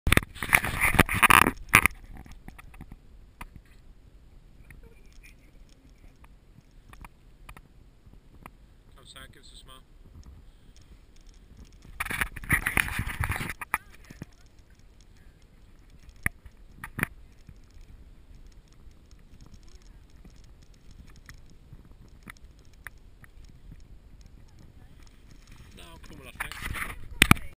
Beacon Hill Chilterns.MP4
Top of Beacon Hill in the Chiltern Hills. Note the Red Kite directly above me in the first frame!